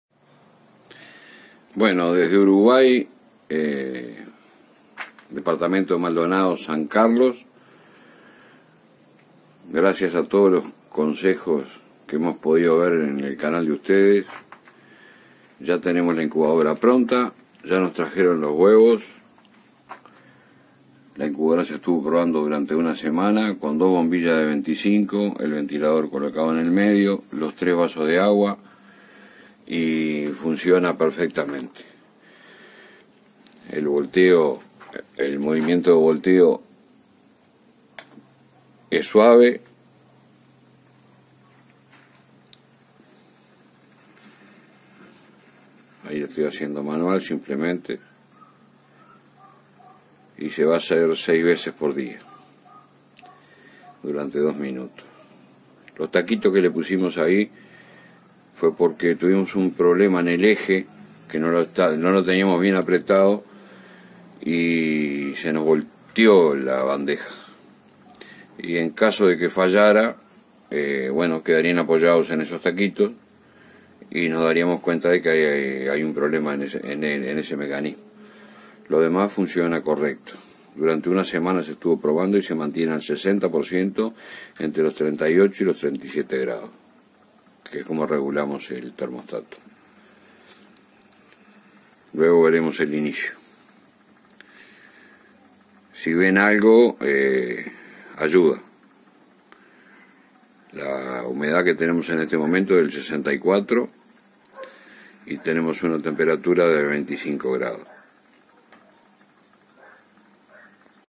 Incubadora casera de huevos en un congelador de frigorífico
Incubadora de huevos casera o artesanal automática con bombillas o focos. Bandeja de volteo de huevos automática del tipo balancín. Una incubadora fácil de hacer y económica o barata.
Este es nuestro correo electrónico